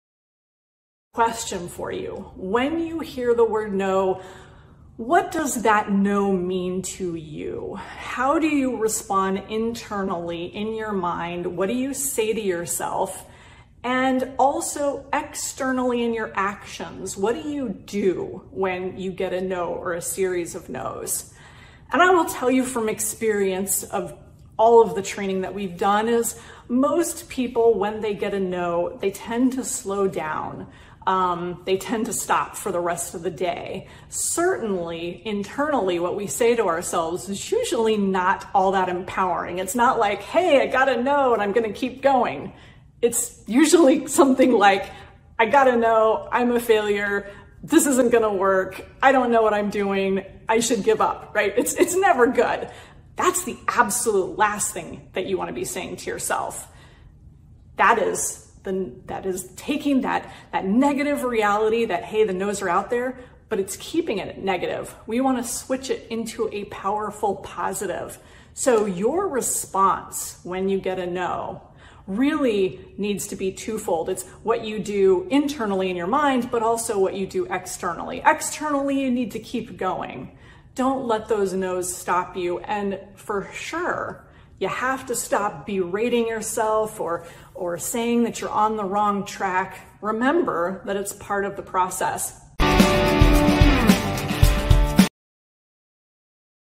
What Does No Mean To You?
When you hear the word, no, what does that "no" mean to you? How do you respond internally in your mind? What do you do externally in your actions? Your response to a no is important for your personal energy and mindset to keep going forward.

FREE QUIZ: Are you curious about your ability to successfully fail? Take the free No-Quotient Quiz and learn about your mindset when it comes to failure, success, and dealing with rejection!

Have you read the book? Grab it on Amazon. (Read a few of the reviews too.)